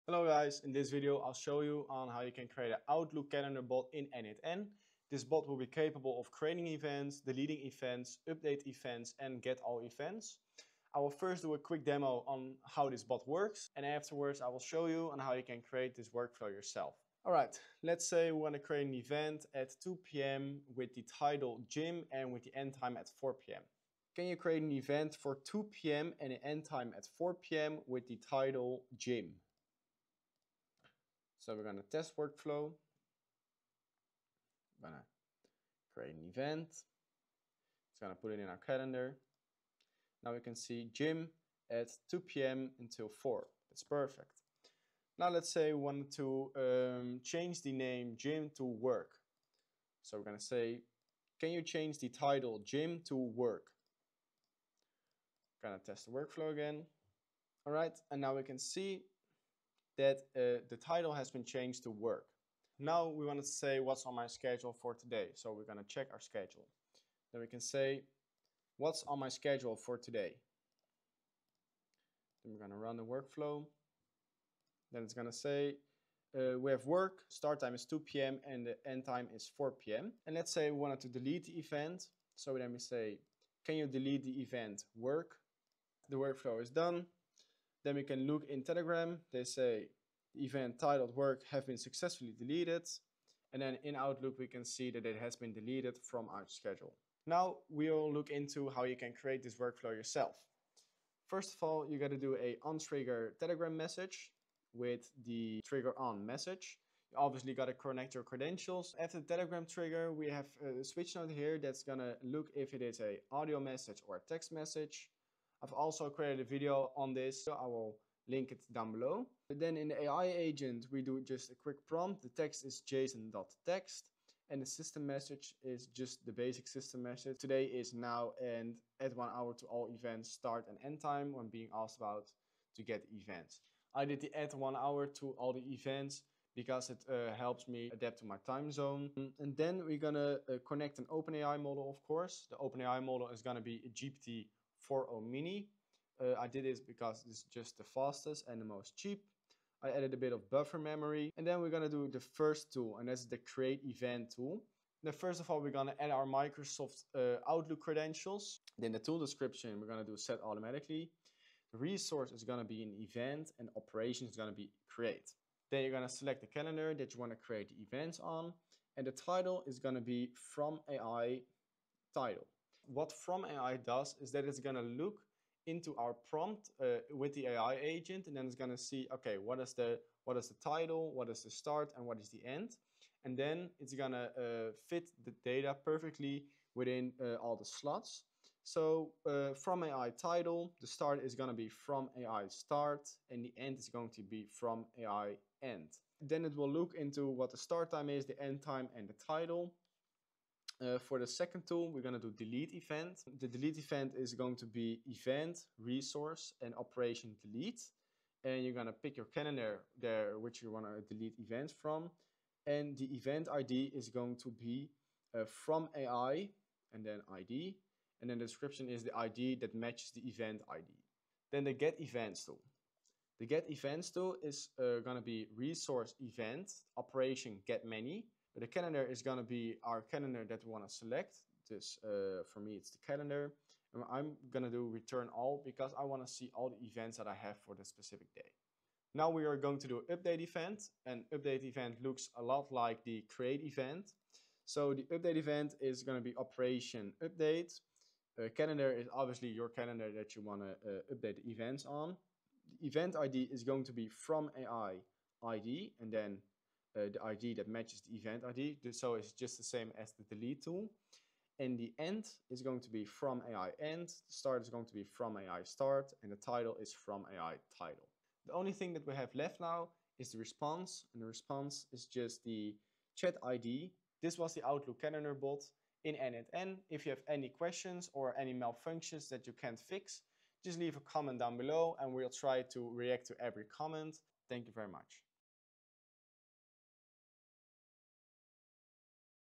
How I fully automated my Outlook calendar using n8n
🚀Join our paid Skool community and go from beginner to expert in n8n and AI Automations! 👇

Automate Outlook Calendar with n8n — trigger from Telegram, parse intent with an AI agent, and create / update / get / delete events via Outlook. This step-by-step build shows credentials, nodes, and the full chat-to-calendar flow.

What you’ll build
-Telegram Trigger to receive commands
-AI agent (From AI) to understand “create/update/delete/get” requests
-Connect Microsoft Outlook Calendar in n8n (OAuth2 / Graph API)
-Actions: Create Event, Update Event, Get Events, Delete Event
-Send confirmations back to Telegram and reuse data in other n8n workflows

Tools used
n8n · Microsoft Outlook  · Telegram nodes

Chapters:
00:00 Intro
00:17 Demonstrating Workflow
01:41 Telegram trigger
02:00 AI agent set-up
02:34 Create event tool
03:22 Delete event tool
03:43 Get events tool
04:04 Update event tool
04:33 Outro